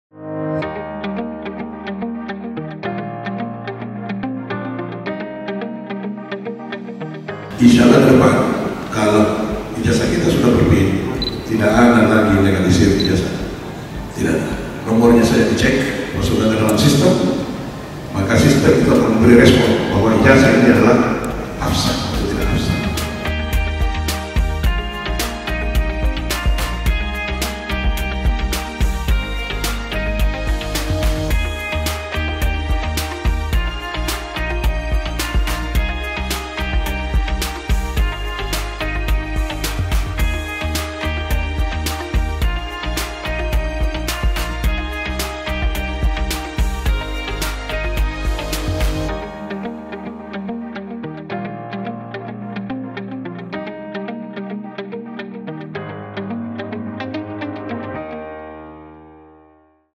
Biro Administrasi Akademik BAA UMI Adakan Bimbingan Teknis Penomoran Ijazah Nasional
Biro Administrasi Akademik (BAA) UMI Adakan Bimbingan Teknis Penomoran Ijazah Nasional (PIN) (26 Desember 2019)